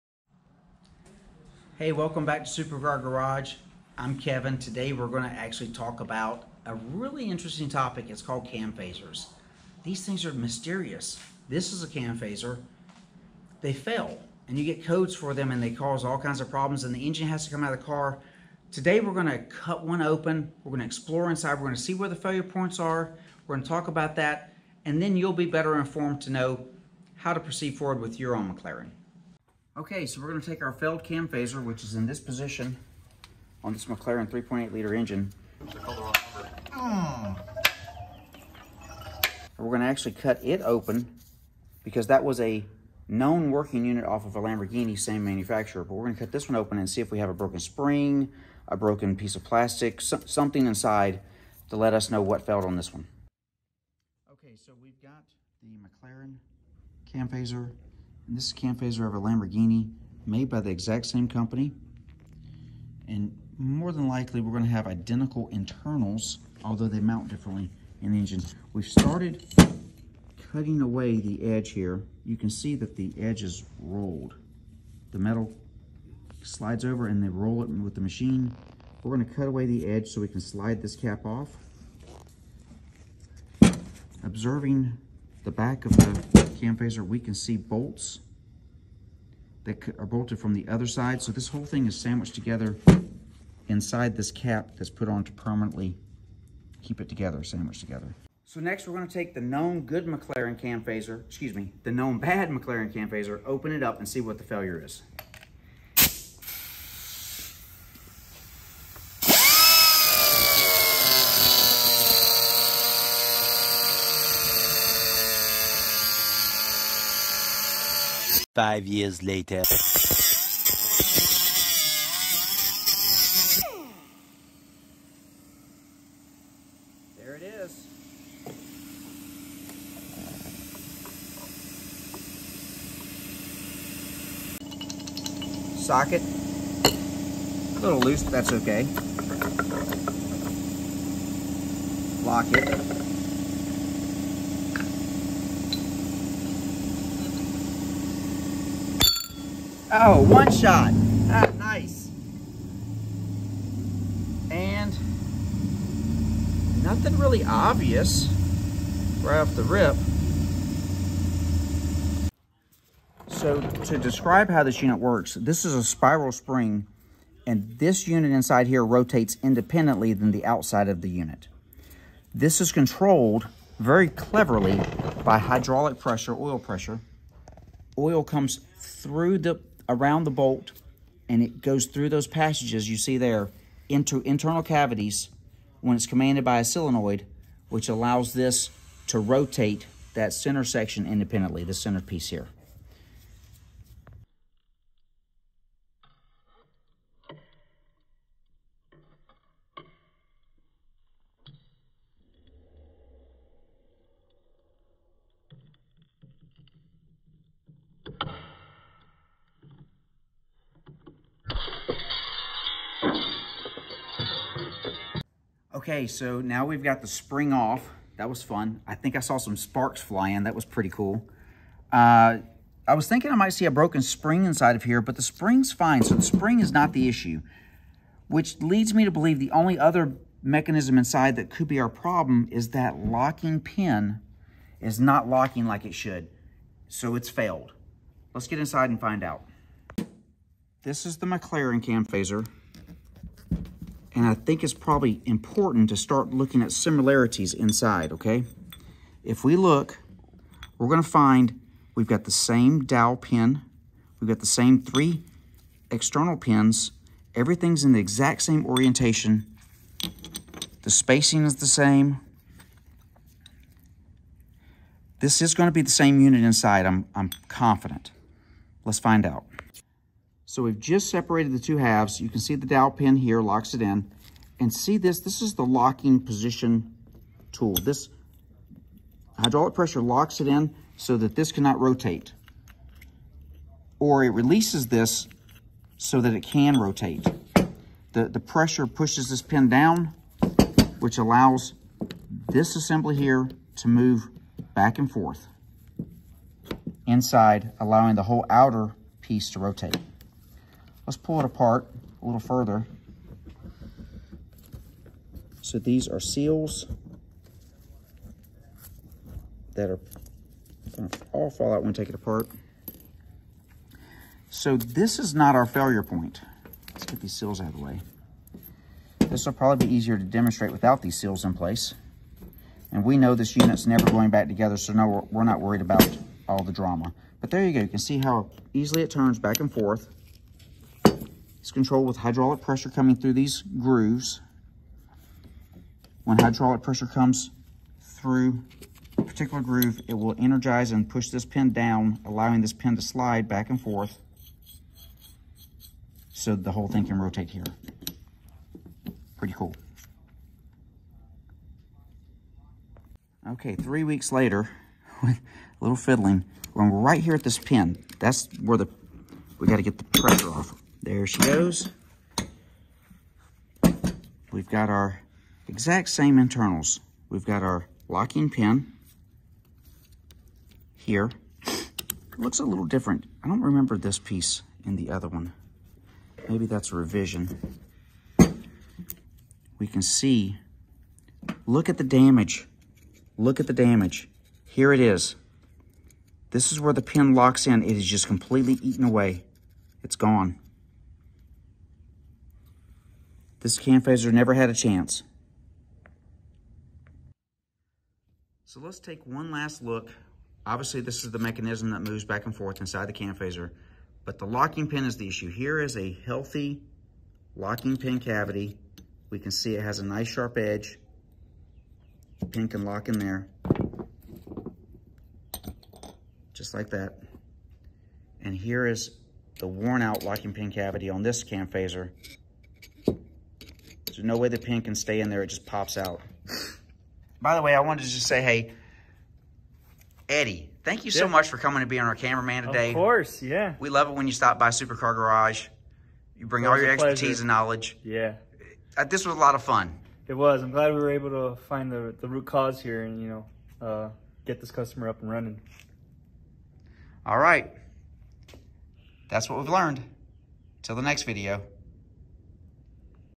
Why Your McLaren Cam Phasers Are Going Bad !
Today we opened up a cam phaser to determine why these things fail.

we provide:
ECU Tuning
PPI's
Repairs
Maintenance
Modifications
Track Prep and much much more...